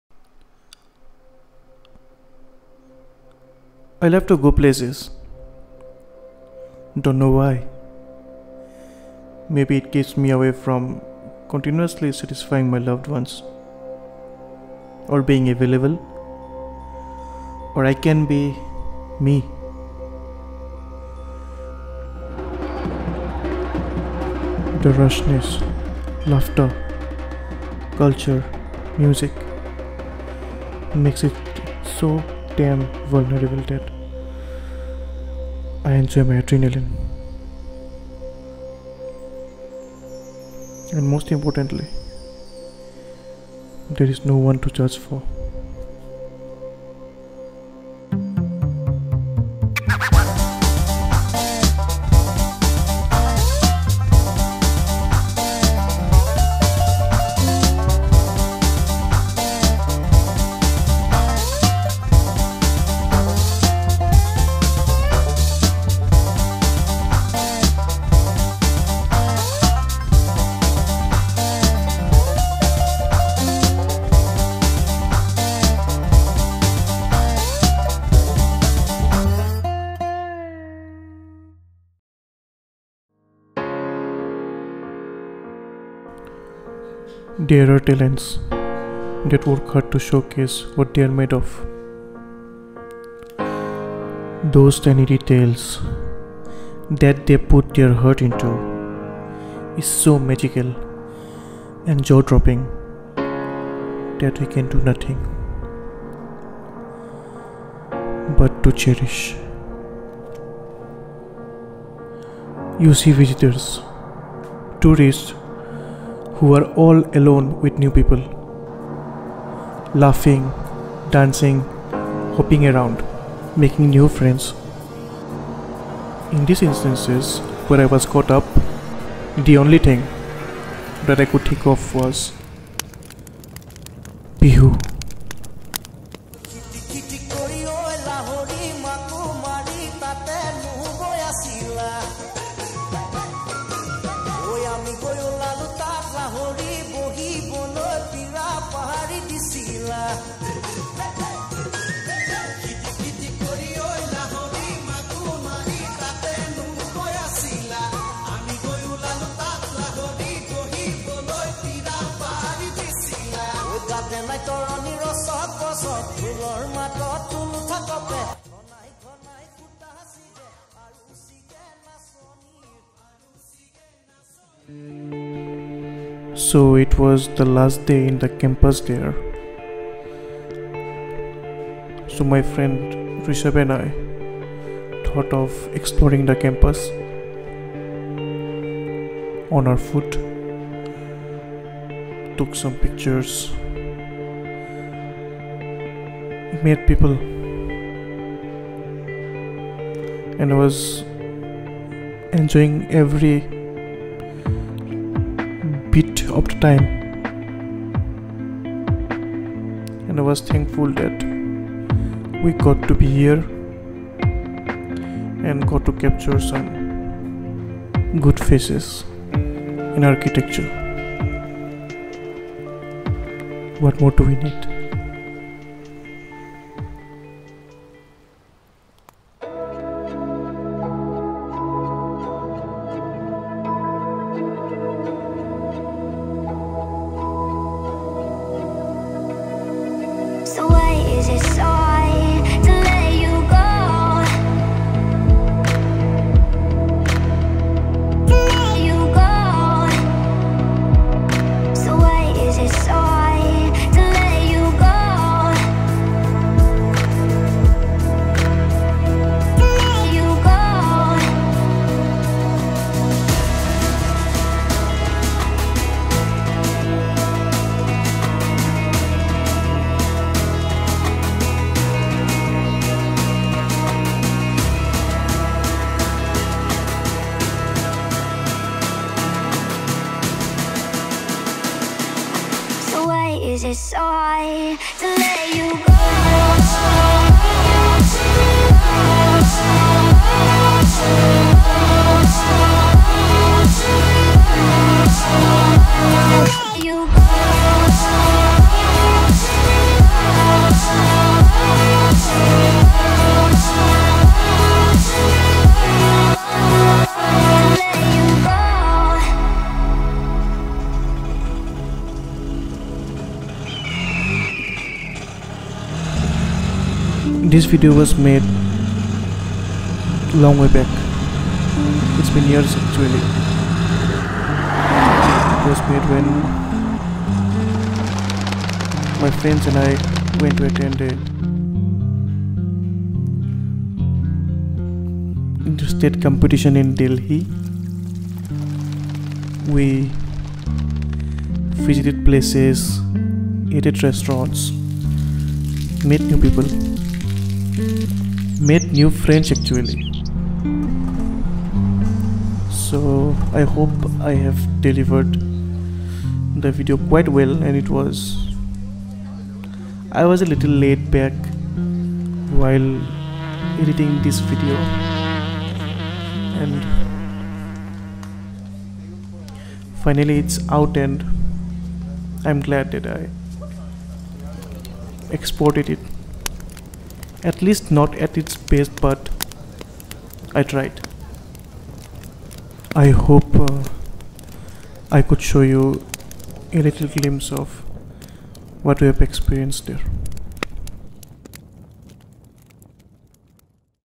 Delhi Travelogue |National Youth Festival | 2018
Filmed this video long back at 2018. The raw files were lingering around. So, Chintu & I tried to combine the clips and trim it down to one. I hope you like it